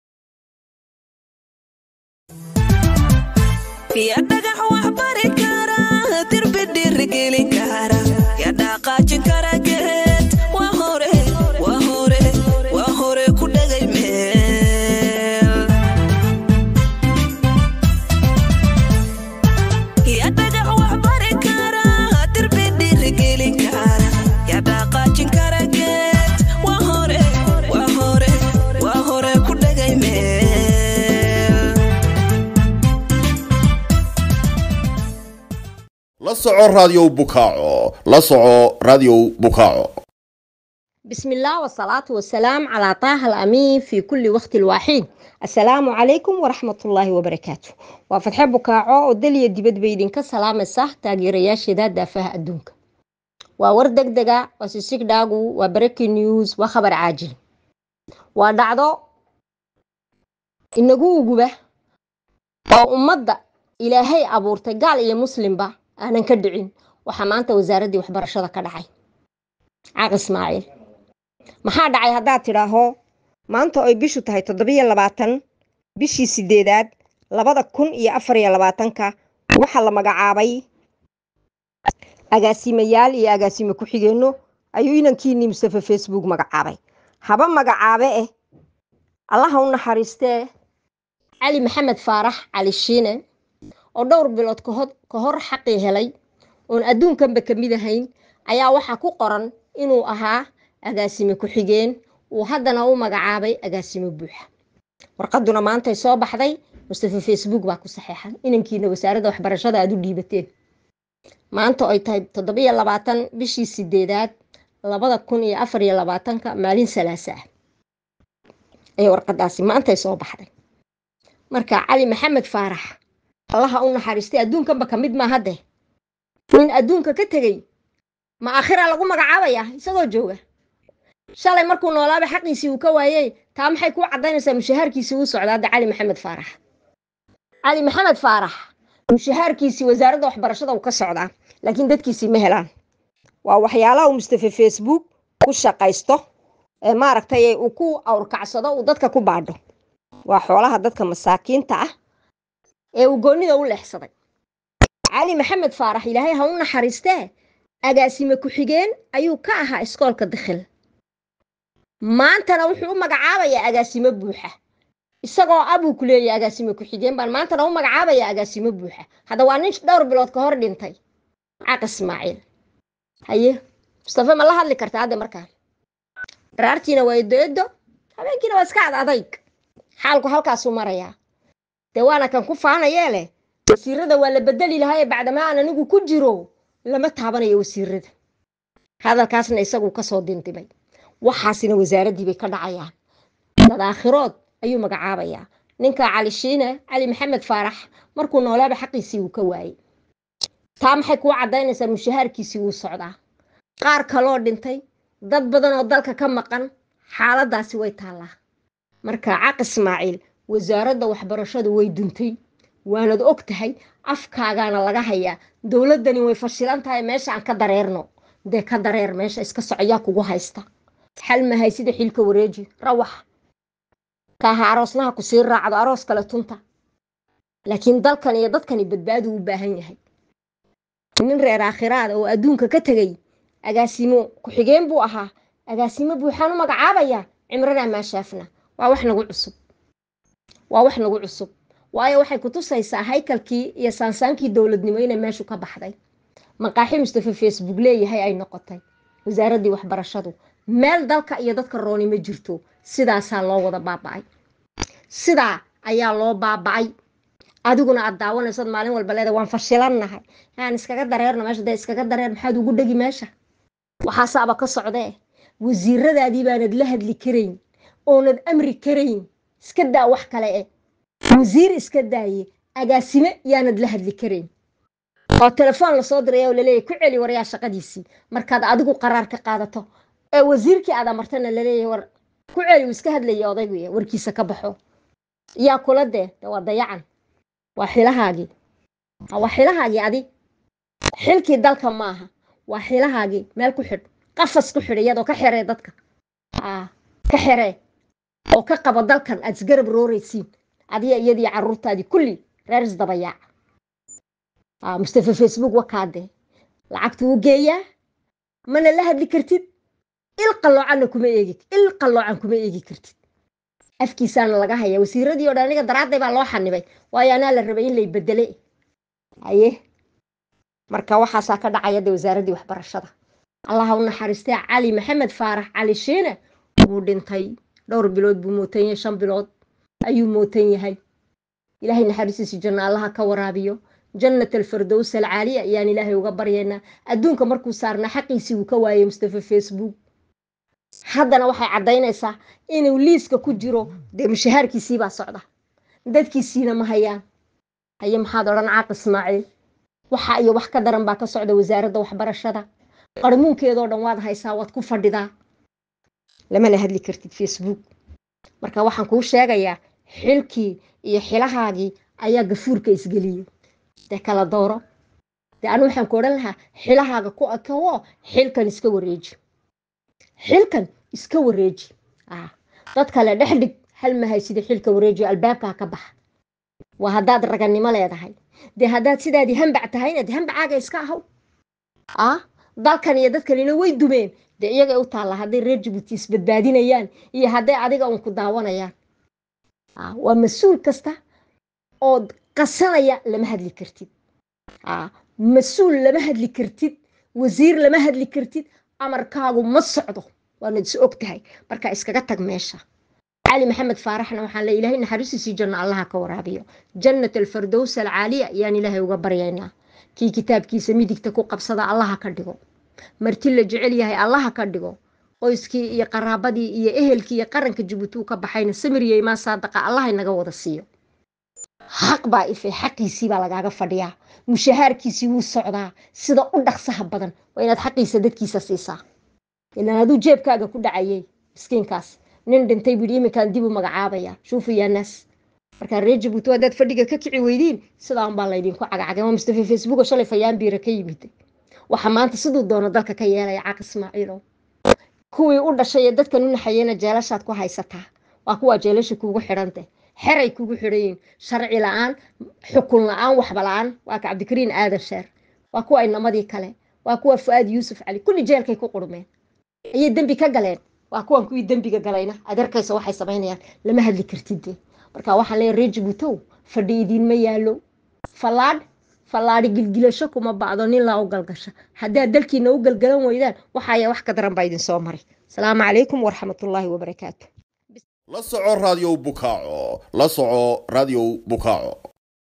🔴 DJIBOUTI 🇩🇯 ➪ Radio Boukao 📻 Édition URGENTE du 27/08/2024 par Fathia Moussa Boukao
Nomination à titre posthume : Djibouti, seul pays où un décédé devient principal d'un collège public.